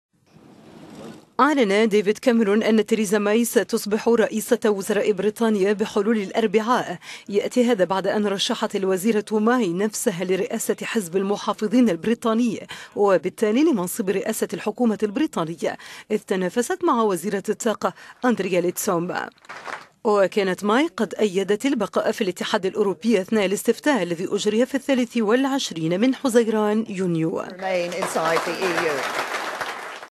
تيريزا ماي رئيسة وزراء بريطانيا بحلول الأربعاء
أعلن ديفيد كاميرون أن تيريزا ماي ستصبح رئيسة وزراء بريطانيا بحلول الأربعاء. يأتي هذا بعد أن  رشحت الوزيرة ماي نفسها لرئاسة حزب المحافظين البريطاني، وبالتالي لمنصب رئاسة الحكومة البريطانية، إذ تنافست مع وزيرة الطاقة أندريا ليدسوم.

وكانت ماي قد أيدت البقاء في الاتحاد الأوروبي أثناء الاستفتاء الذي اجرى في 23 حزيران/يونيو، فيما تعهدت بعد صدور نتائج الاستفتاء، بالعمل على توحيد دعاة البقاء في الاتحاد الأوروبي ودعاة الخروج …